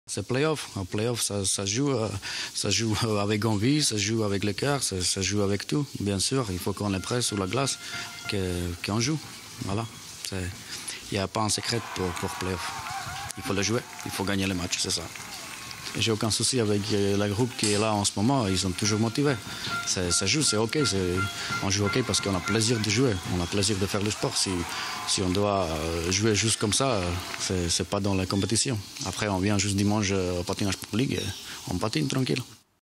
Hogly vs Boulogne-Billancourt (9-3) : Interview Juraj Ocelka
Sites webs/blogs, copiez cette vidéo avec le bouton "Intégrer" !
 Interview de Juraj Ocelka, entraineur des "Aigles" du Hogly – La Roche-sur-Yon sur la belle victoire de son équipe 9-3 face à Boulogne-Billancourt.